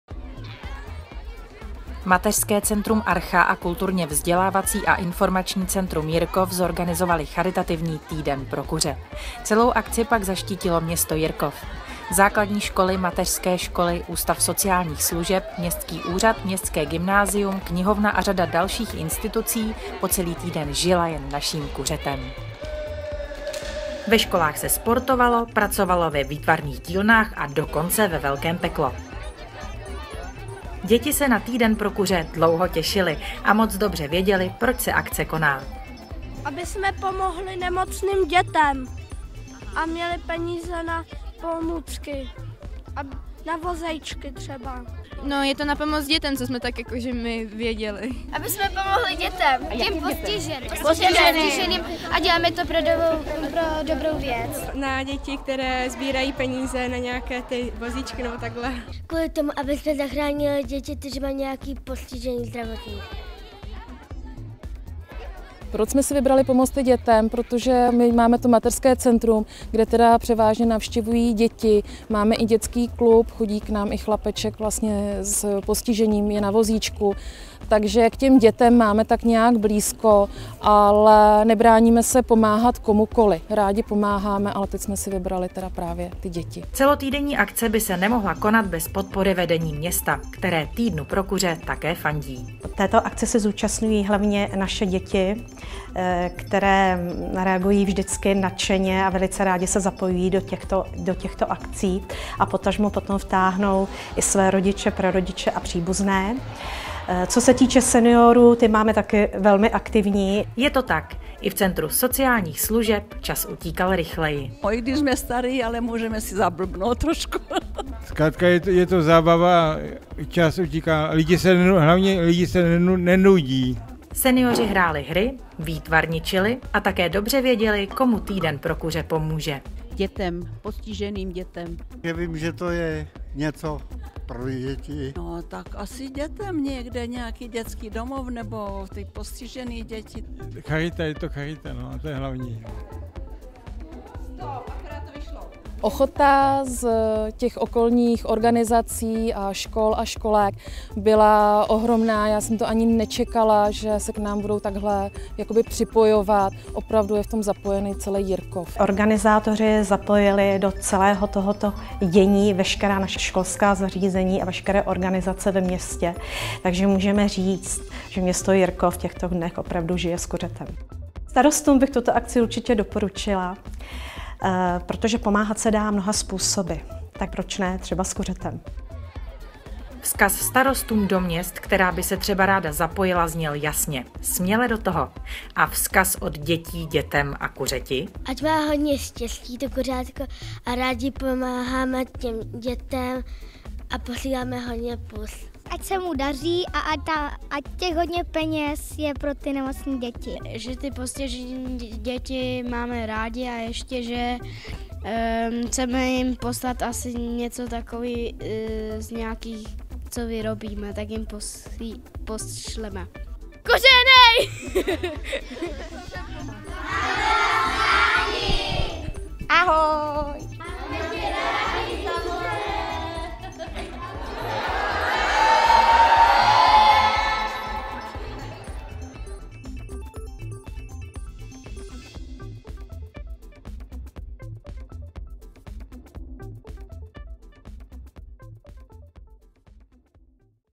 Týden s kuřetem v Jirkově
Mateřské centrum Archa a Kulturně vzdělávací a informační centrum Jirkov uspořádaly celotýdenní charitativní akci Týden pro kuře. Podívejte se na reportáž, co vše se v Jirkově konalo a kdo pomohl.

hudba: Eva Hosnedlová